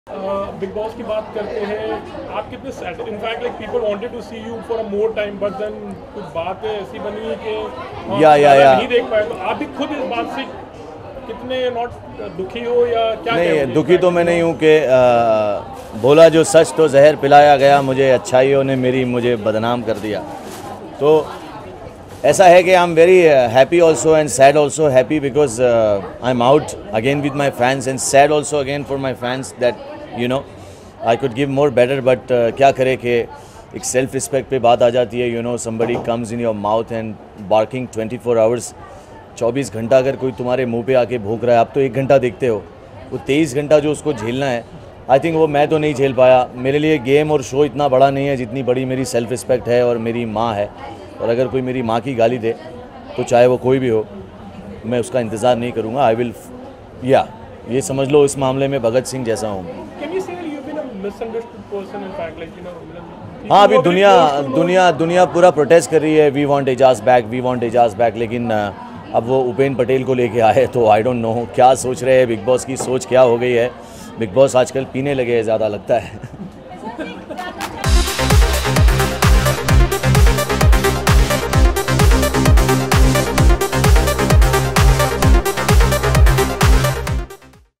Drunk Ajaz Khan talks about Bigg Boss Halla Bol CONTESTANTS
Ek number manaas Ajaz Khan who was thrown out of Bigg Boss Halla Bol for his violent behaviour spoke about Bigg Boss Halla Bol contestants. Not his usual self, he looked quite drunk